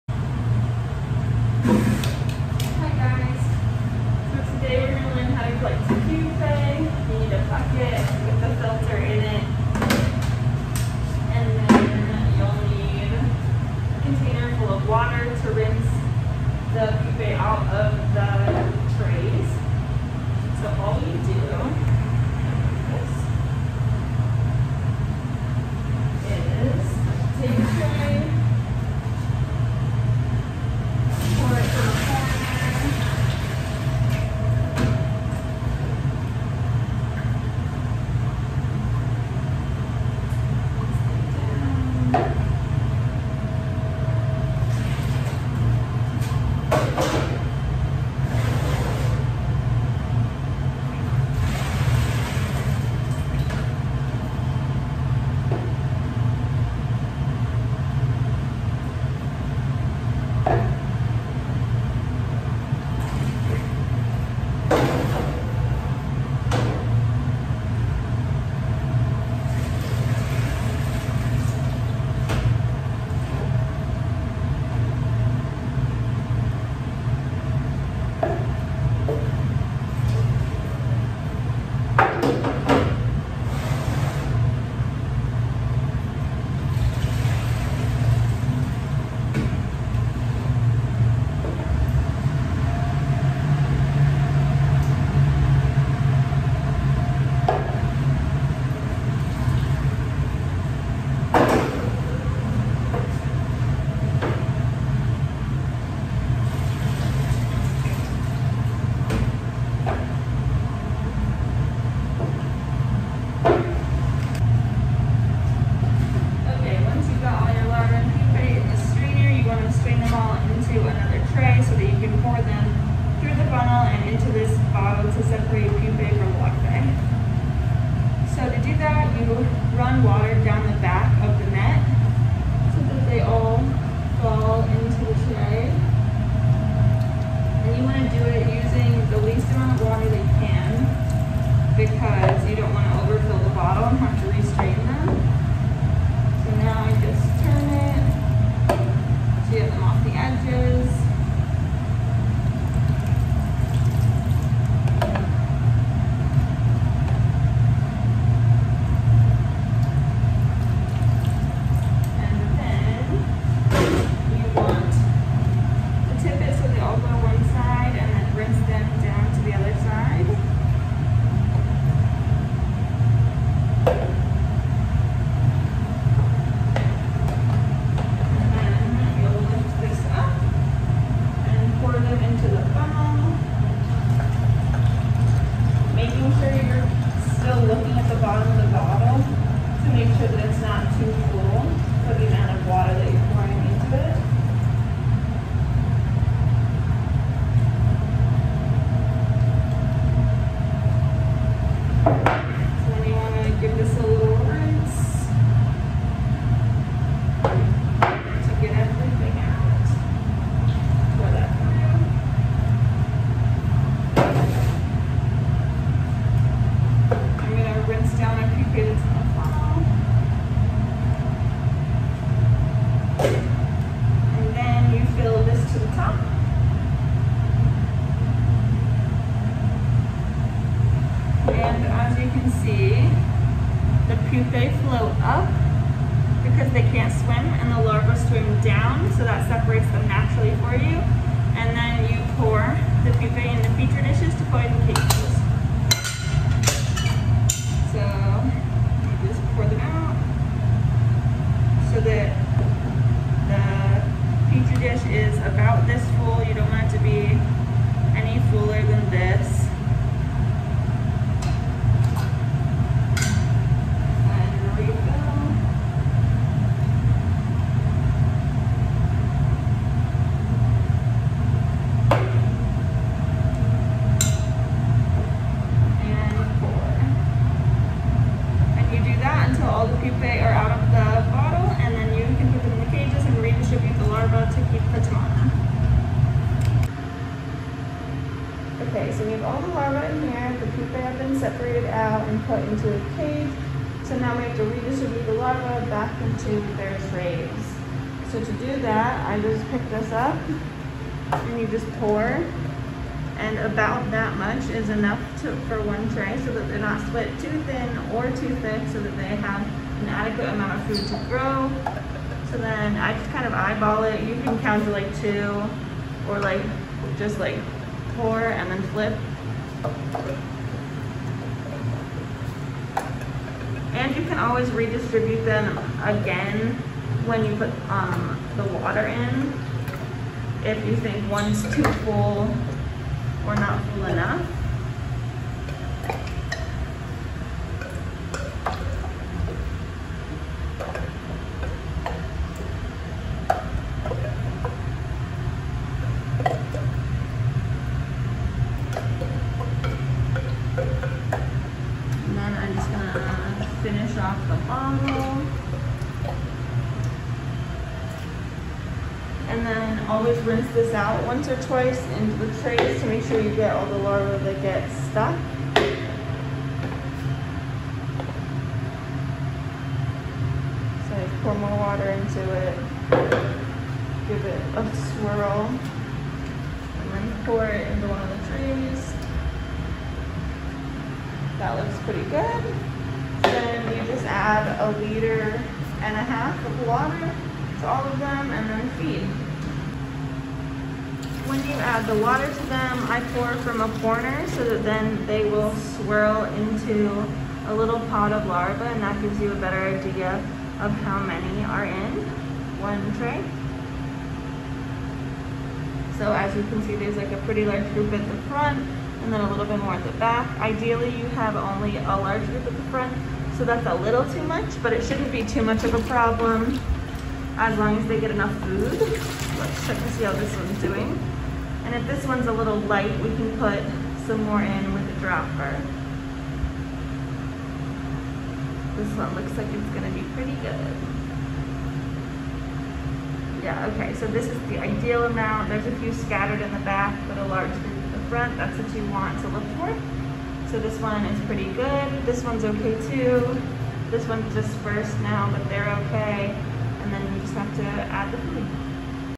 Straining larva, collecting pupae, and redistributing large larva
Each week day pupae are collected and put in the metal cage to maintain adult mosquito numbers, and larva need to be given fresh water so they are healthy and able to grow into pupae